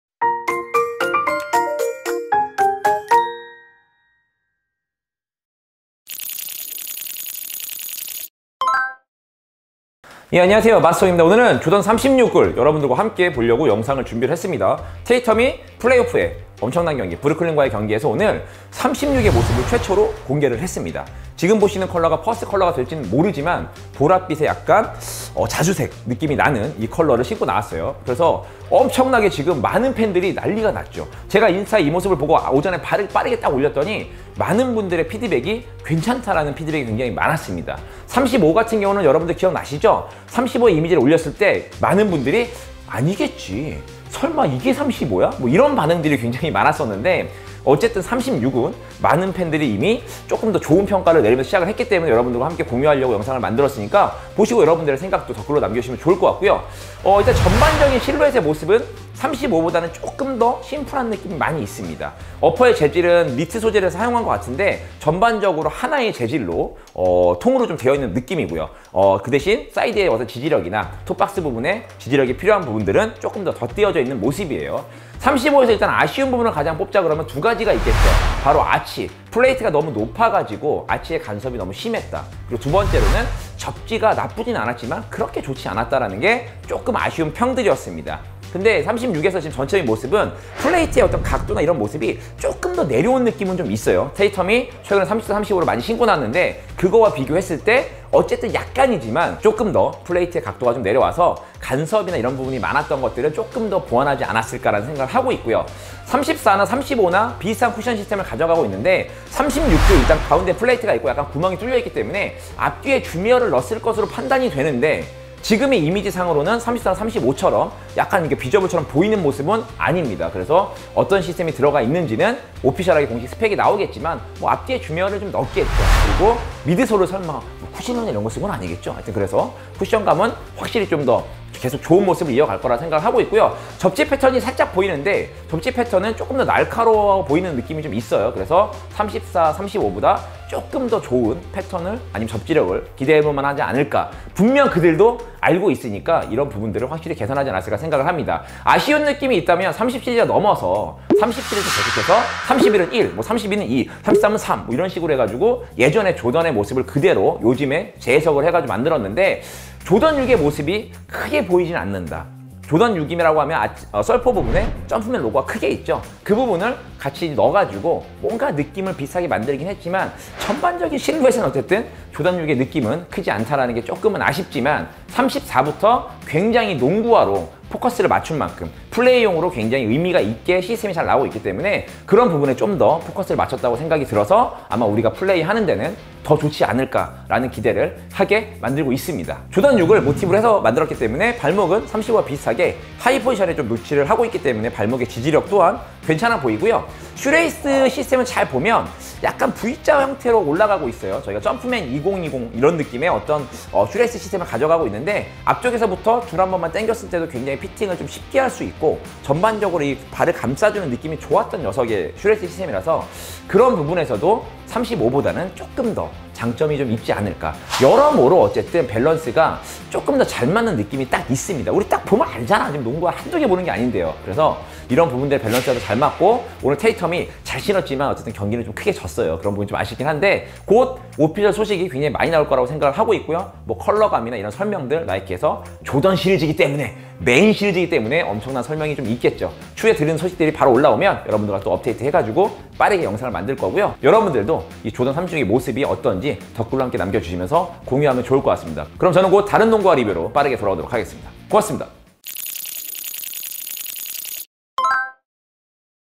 이걸 이렇게 공개한다고?? 에어 조던36 공개!! (AIR JORDAN 36)
에어조던 메인 시리즈 36이 테이텀을 통해 오늘 공개가 되었습니다.
느낌은 상당히 오네요 여러분은 어떻신가요?